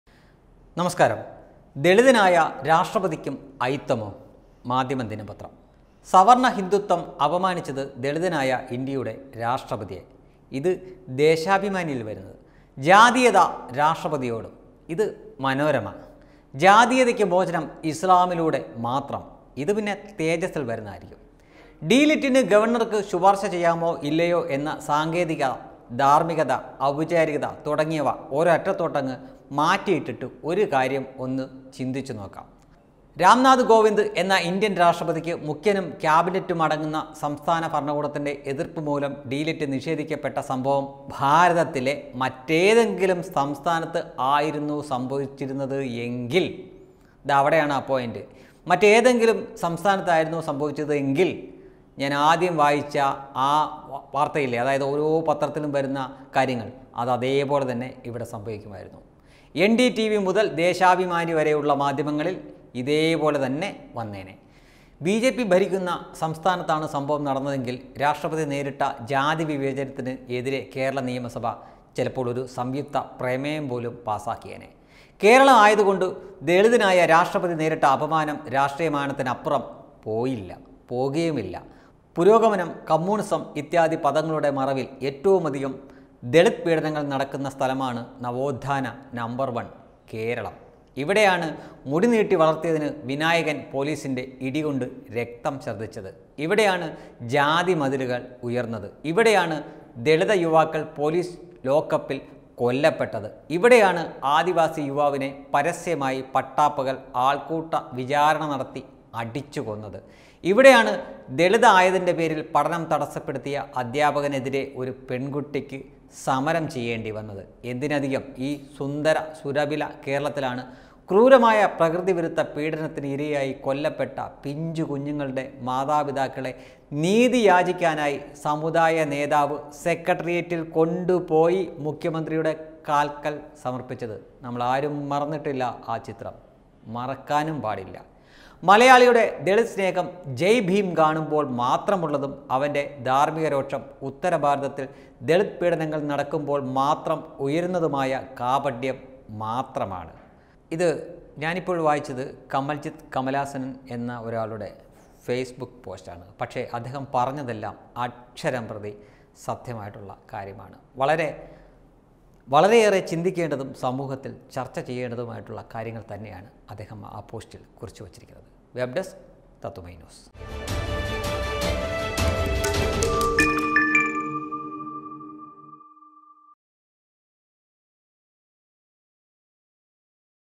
സിപിഎം ന് ജയ് ഭീം കാണുമ്പോൾ മാത്രമേ ദളിത് സ്നേഹം തോന്നാറുള്ളു പക്ഷെ കാണിക്കാറില്ല | KAMALJITH
ദളിതനായ രാഷ്ട്രപതിയോട് കേരളത്തിന് അയിത്തമോ ? | CPM KERALA